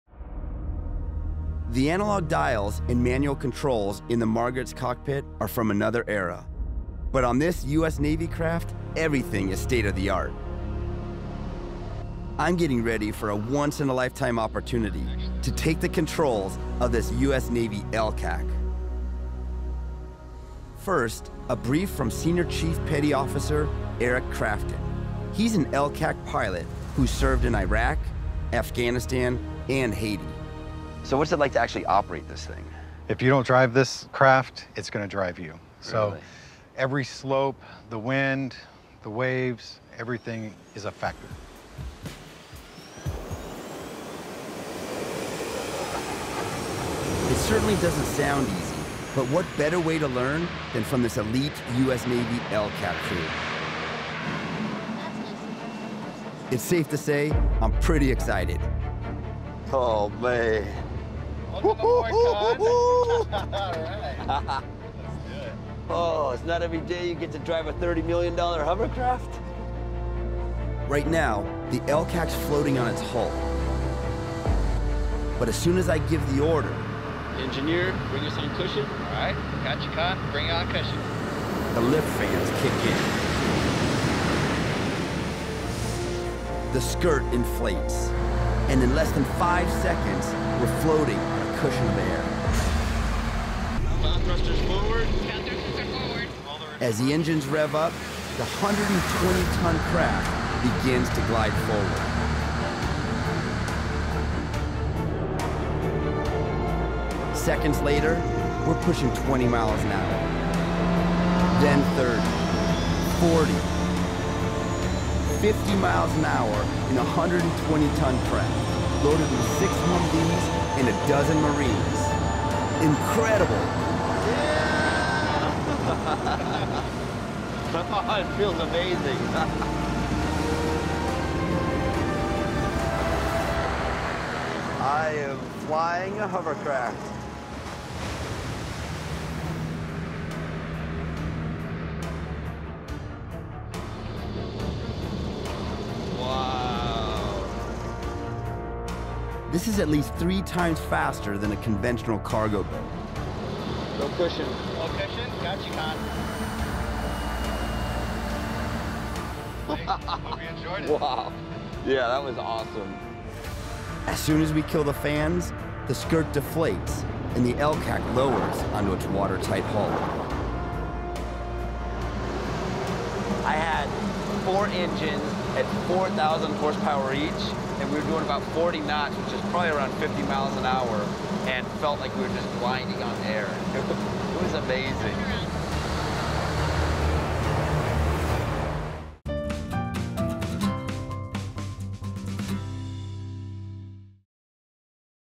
The Only Way to Really Understand a Hovercraft is to Drive One
What’s it like to operate a giant hovercraft? There’s no better person to ask than a U.S. Navy pilot who’s driven the LCAC on tours of duty in Iraq, Afghanistan, and Haiti.